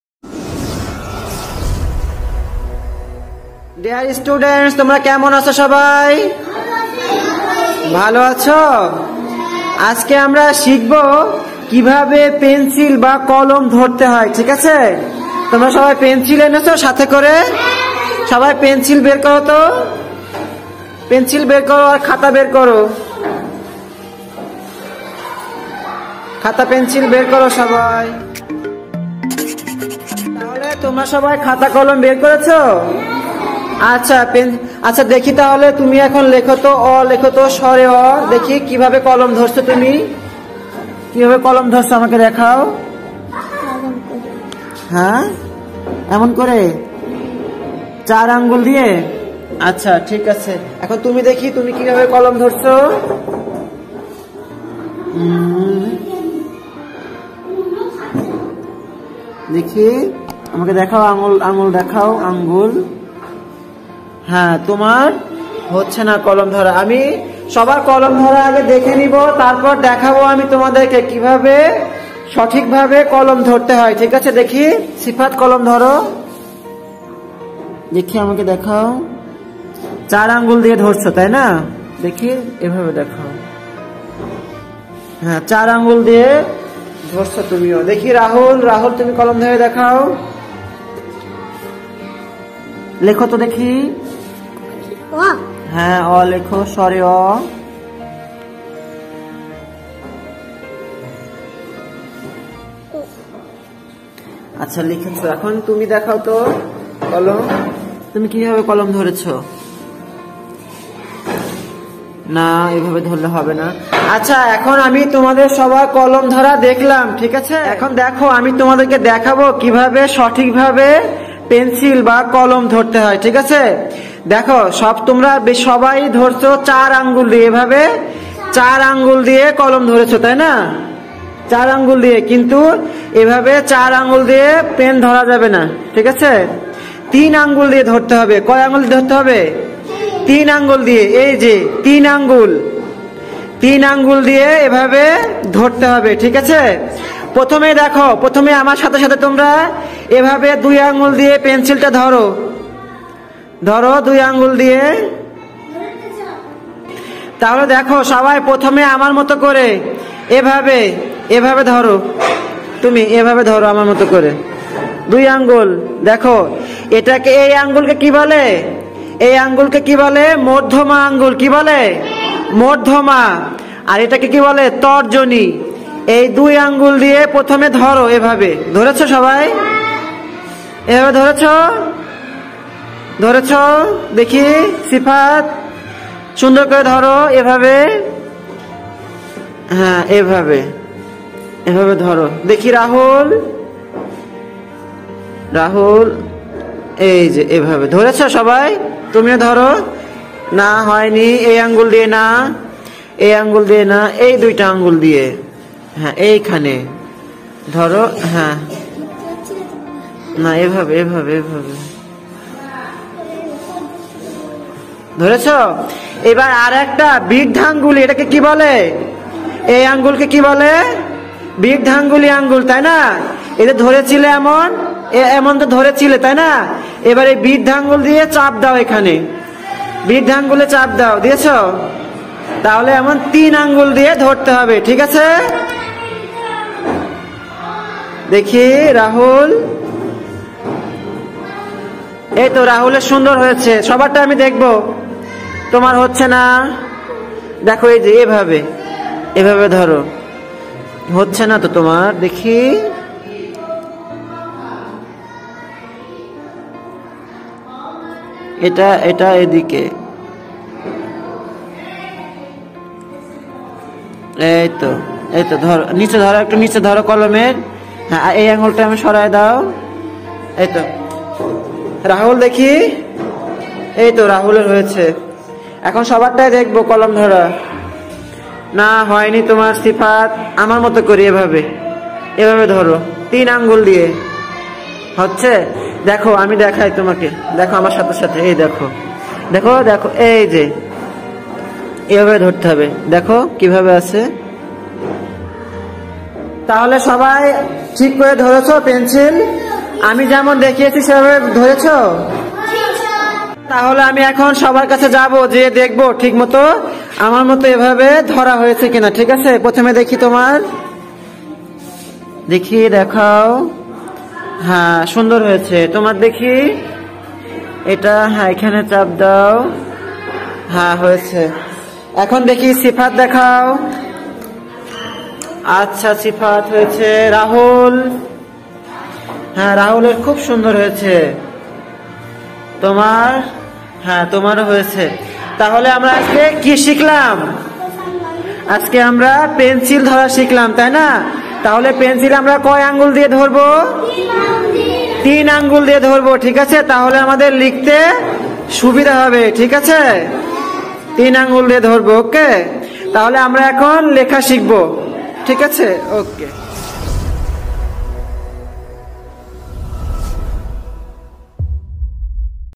লেখার হাতেখড়ি | সঠিকভাবে কলম ধরার নিয়ম | Right way to holding pen | bd primary school activity
মাঝে মাঝে এভাবে খেলার সাথে পাঠদান করালে আমার মনে হয় ছাত্র-ছাত্রীরা পড়ালেখায়  অনেক বেশি  আগ্রহী হবে।

kids school activity.